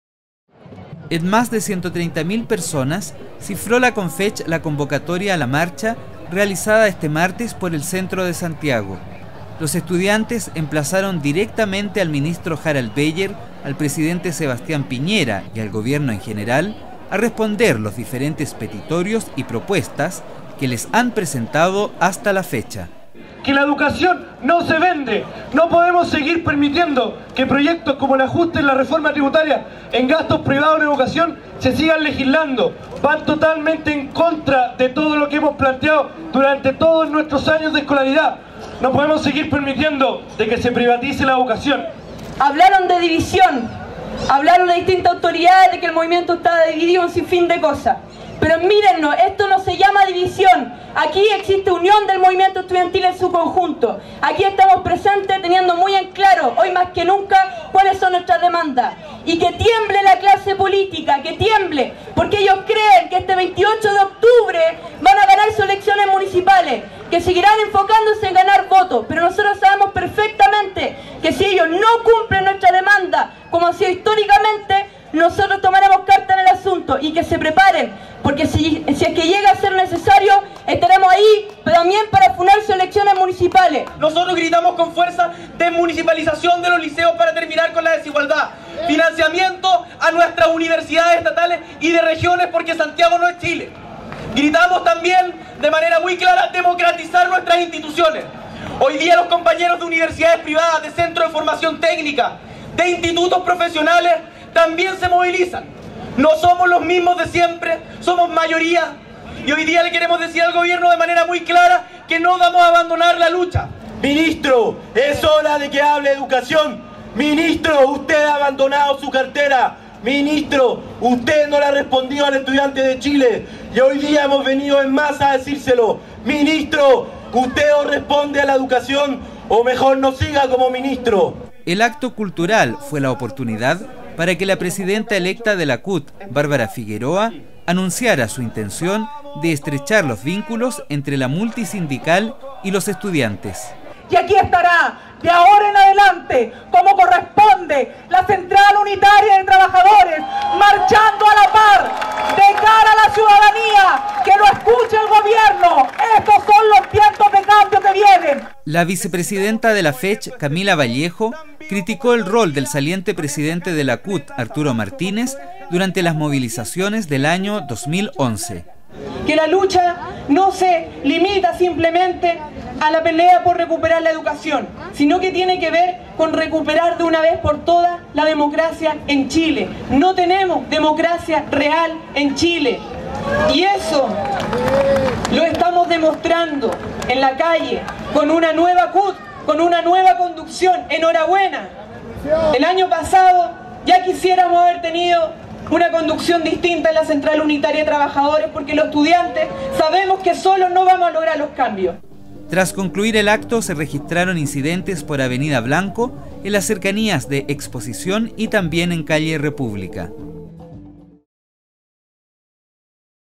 La gran marcha del 28 de agosto
Resumen de la marcha estudiantil que convocó a más de 130 mil personas en la capital este 28 de agosto. 28/08/2012.